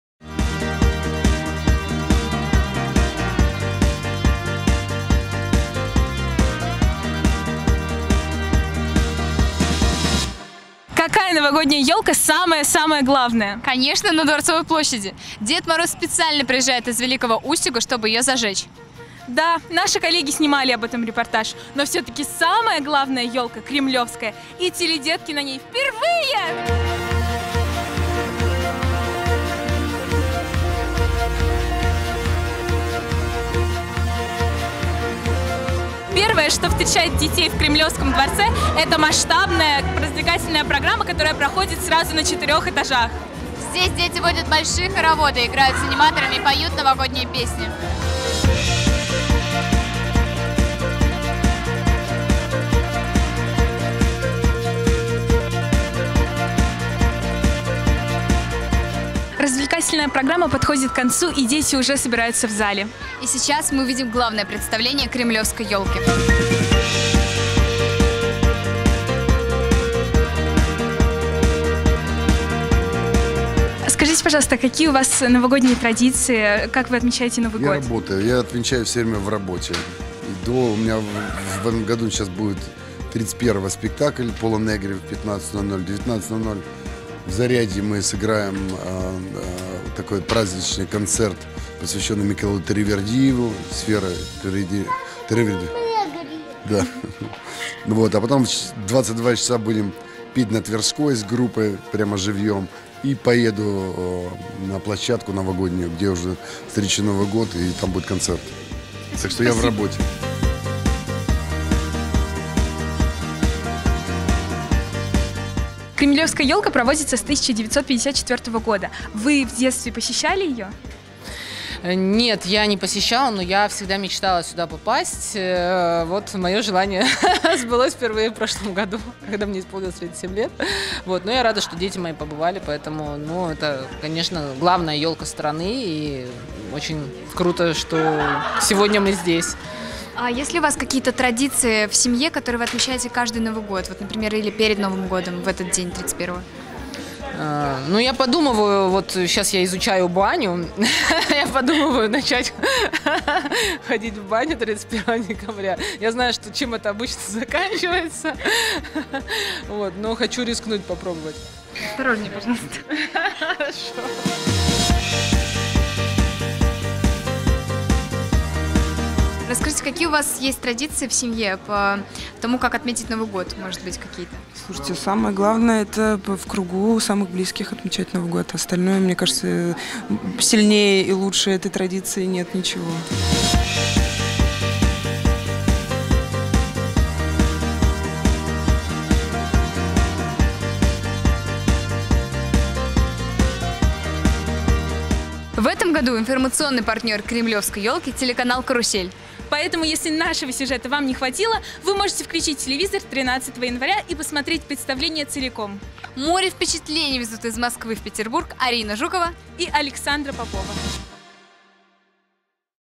Кремлёвская ёлка - 2019
Теледетки побывали на Главной ёлке страны в Государственном Кремлёвском дворце. Большая красочная развлекательная программа ждала детей сразу на четырех этажах, а в главном зале был представлен интересный спектакль. Среди гостей звезды с детьми - Филипп Киркоров, Лена Темникова, Гоша Куценко, Елена Борщевская и многие другие.
Все подробности - в сюжете наших корреспондентов Саши Поповой и Арины Жуковой.